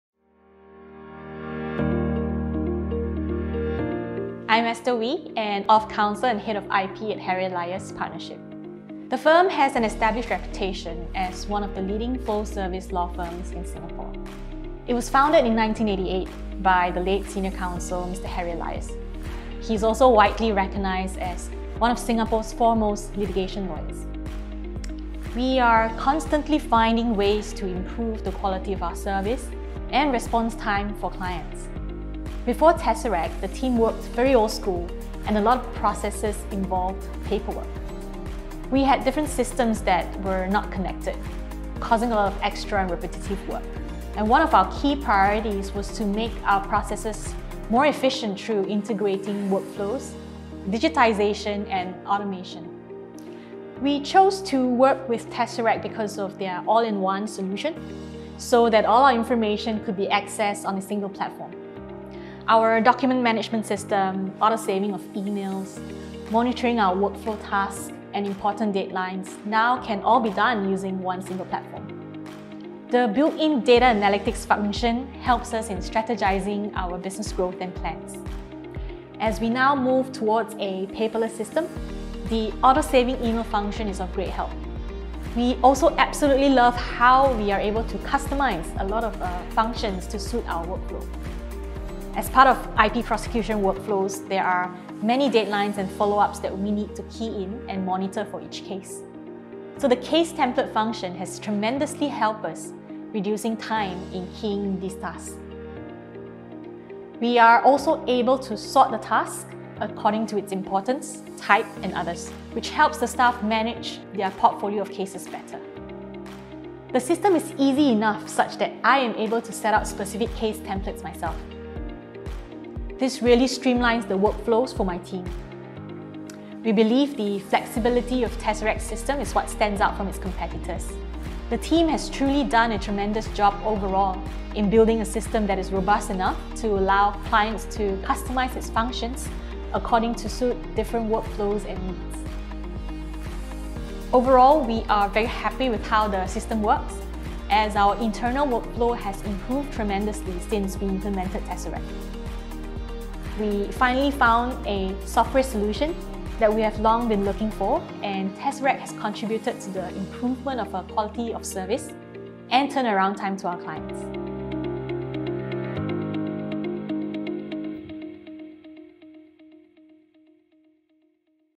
Harry Elias Partnership LLP X Tessaract.io | Business Productivity | Document Management
Esther Wee, Head of IP at Harry Elias Partnership LLP talks about the benefits of workflow automation and how Tessaract.io’s all-in-one cloud-based software solution has contributed to the growth and success of the IP team. It can be so hard to keep track of thousands of files especially when they are paper based, this also takes a lot of filing space too. The great thing is with Tessaract.io it is all digital, no need to worry about paper. Also documents can be easily named and also put into folders so everything is well organized.